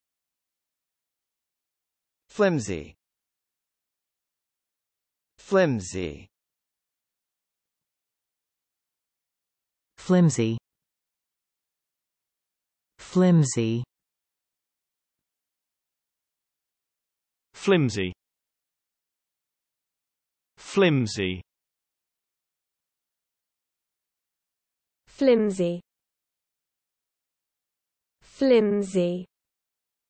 How to pronounce flimsy
Learn how to say Flimsy in English correctly with TextToSpeech.io free pronunciation tutorials. American & British English pronunciation of male & female voices provided in videos. Check them out!

    Flimsy definitions:
    (n): a thin strong light-weight translucent paper used especially for making carbon copies
    (s): having little substance or significance; "a flimsy excuse"; "slight evidence"; "a tenuous argument"; "a thin plot"
    (s): lacking solidity or strength; "a flimsy table"; "flimsy construction"; "a fragile link with the past"
    (s): resembling cardboard especially in flimsiness; "apartments with cardboard walls"
    (s): very thin and insubstantial; "thin paper"; "flimsy voile"; "light summer dresses"